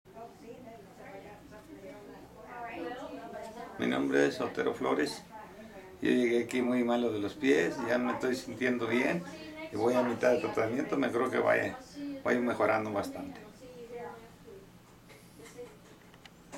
Spanish: Leg cramps when walking.
Mr. Sotereo could not walk or stand for more than 10 min without leg cramps. He is now able to walk and stand without much pain or cramps. :)